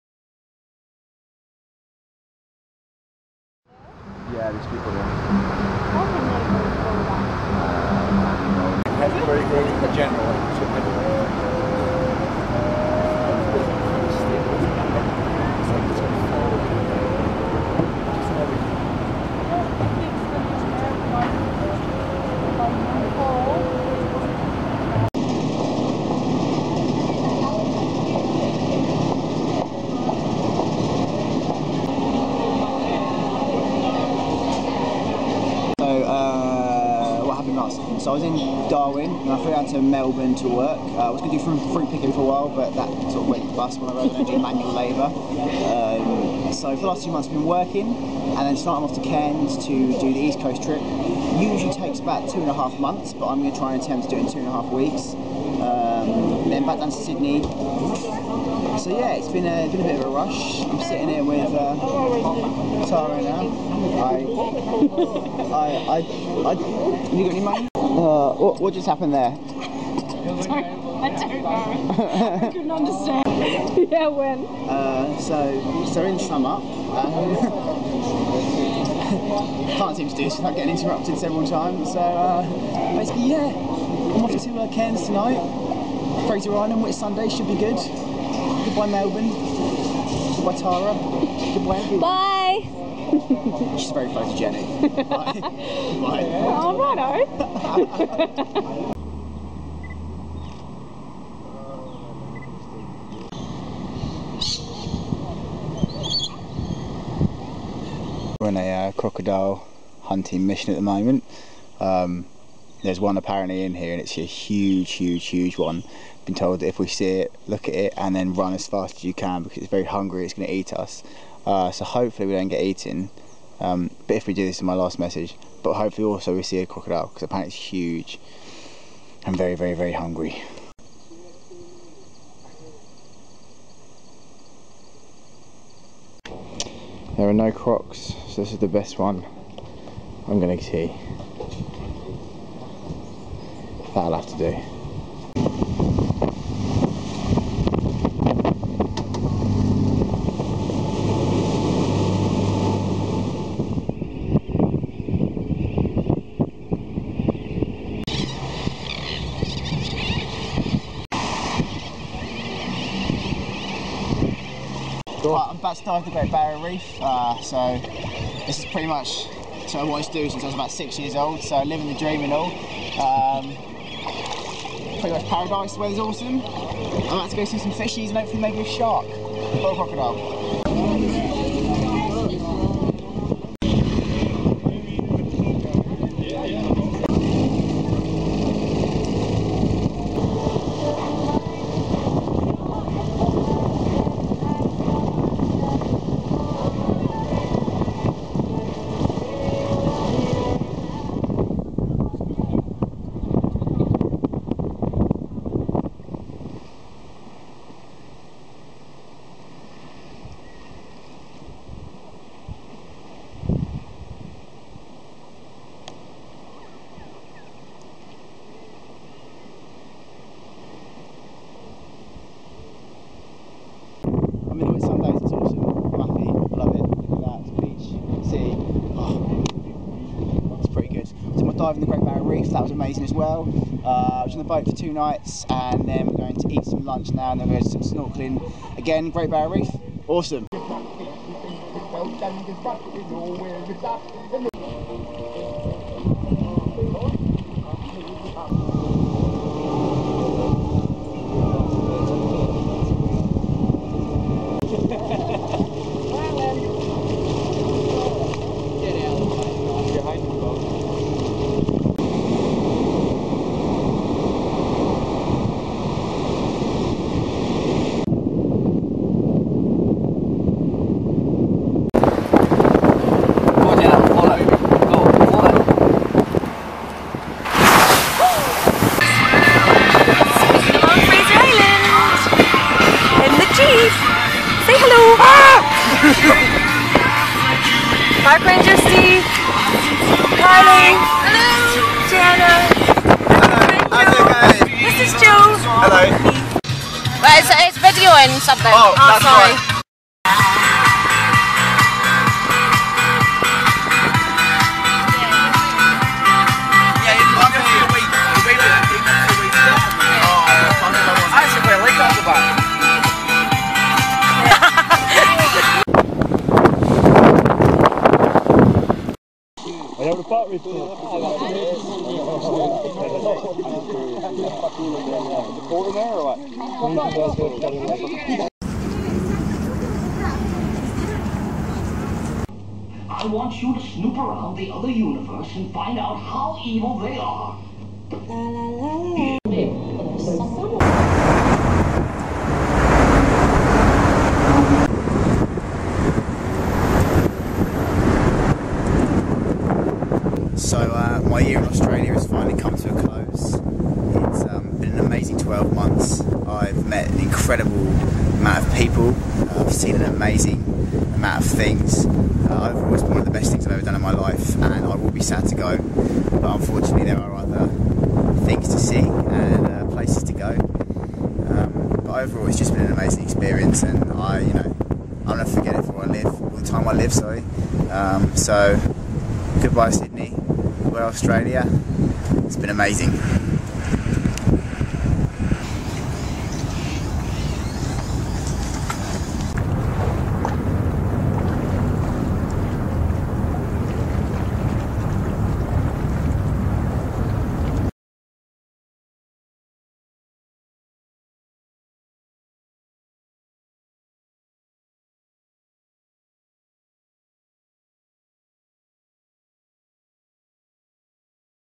Paradise Found: The East Coast of Australia
Backpacking the East Coast of Australia between August and September 2012, taking in The Great Barrier Reef, The Whitsundays and Fraser Island ending up in Sydney at the end.